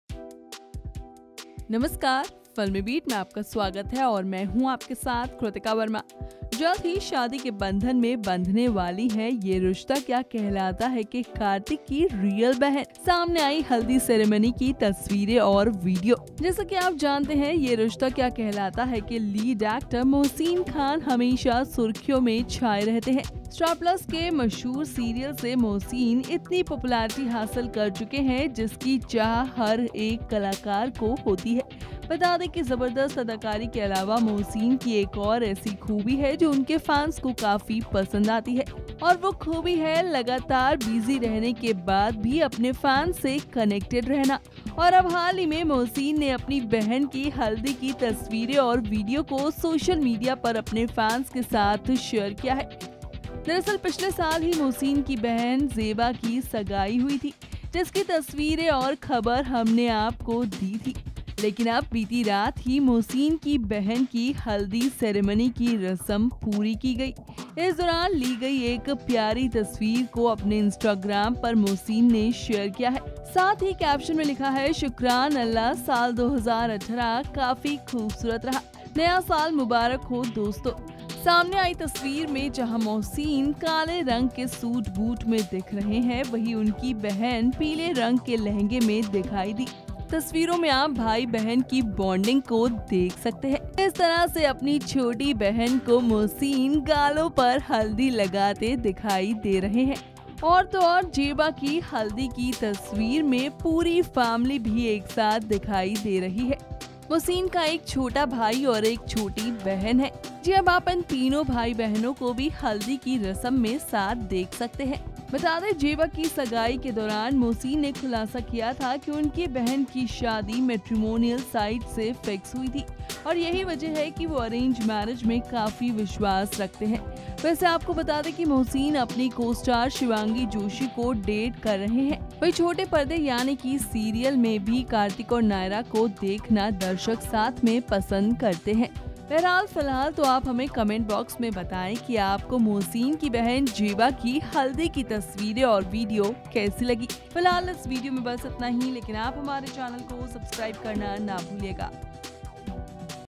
Mohsin Khan's sister Zeba's Haldi ceremony, photos goes VIRAL | FilmiBeat
Mohsin Khan's sister Zeba's Haldi ceremony, photos goes viral. TV actor Mohsin Khan, who is currently playing the lead role of Kartik Goenka in top-rated & long-running Star Plus show Yeh Rishta Kya Kehlata Hai opposite Shivangi Joshi.